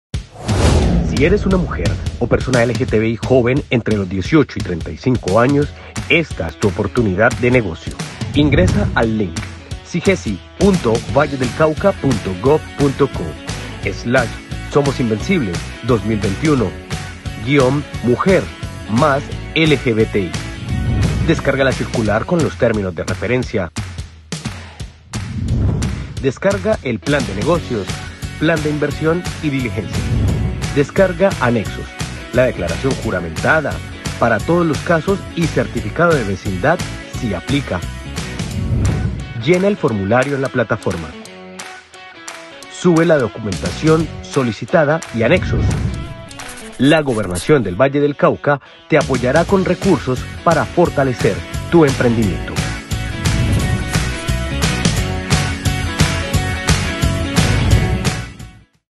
SOMOS INVENCIBLES 2021
Atención Emprendedores de la comunidad LGTBIQ+ y mujeres residentes en Yotoco, la Gobernación Del Valle del Cauca los acompaña en sus proyectos de emprendimiento, únete a la actualización, suscribe tus datos, solicita apoyo y recibe herramientas para fortalecer tu emprendimiento. Sigue detenidamente la información en el siguiente video, también puedes acercarte a la Secretaría de Desarrollo Social y Talento Humano de Yotoco, para mayores informes.